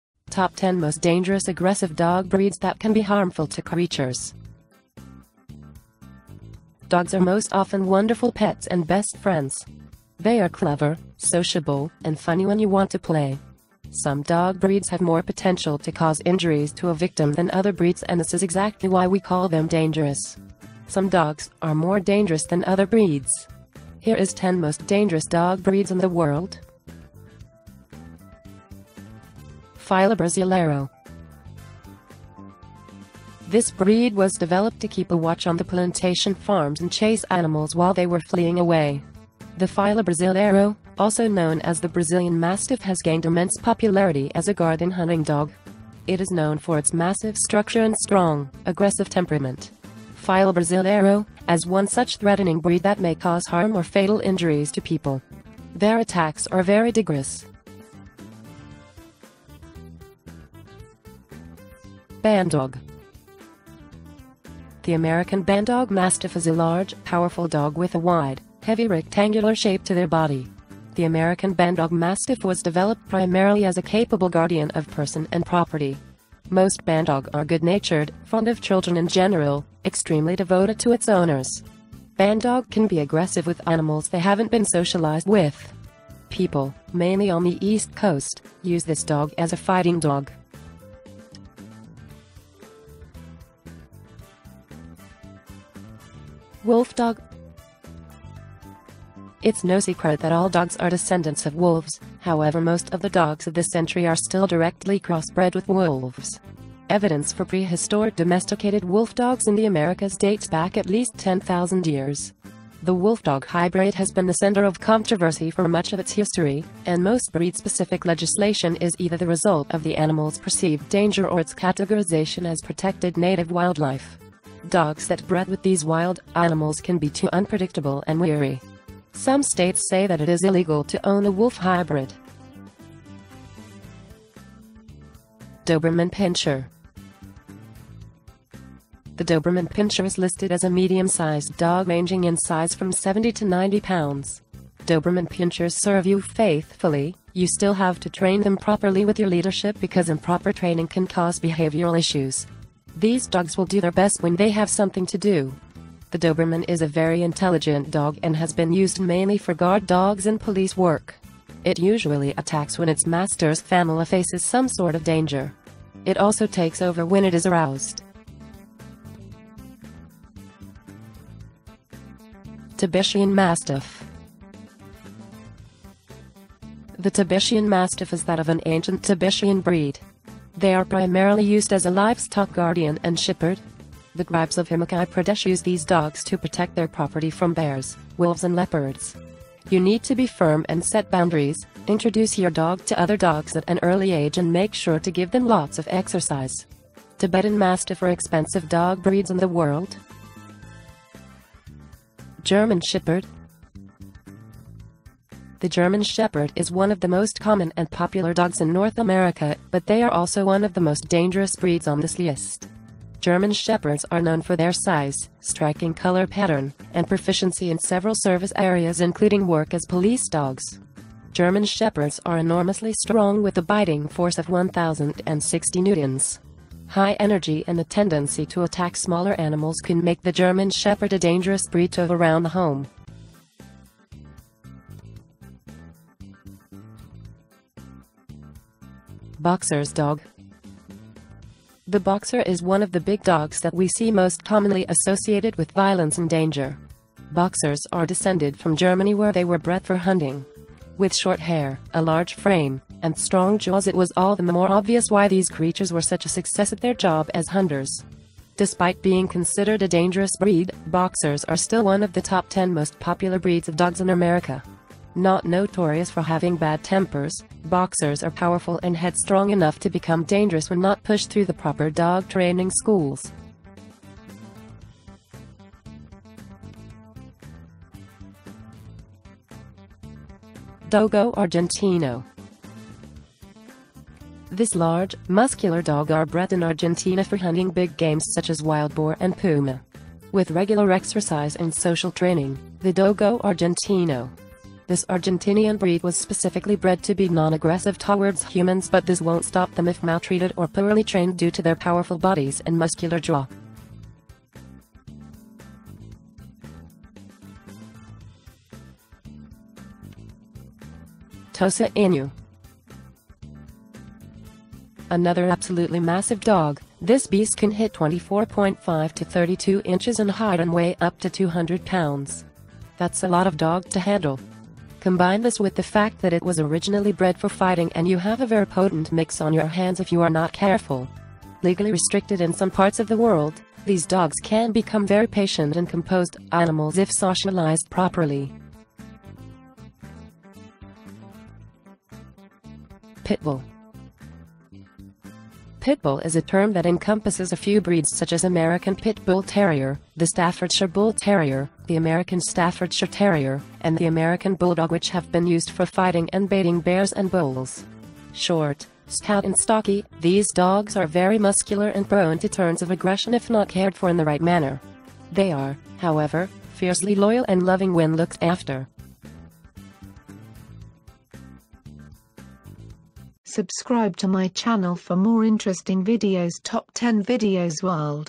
Top 10 Most Dangerous Aggressive Dog Breeds That Can be Harmful To Creatures
Dogs are most often wonderful pets and best friends. They are clever, sociable, and funny when you want to play. Some dog breeds have more potential to cause injuries to a victim than other breeds and this is exactly why we call them dangerous.  Some dogs are more dangerous than other breeds. Here is 10 most dangerous dog breeds in the world.
Tibetan Mastiff
 German Shepherd
 Pit Bull Terrier
 bandog
 wolfdog
 Dogo argentino
 Fila Brasileiro
 Boxer
 Doberman Pinschers
 Tosa Inu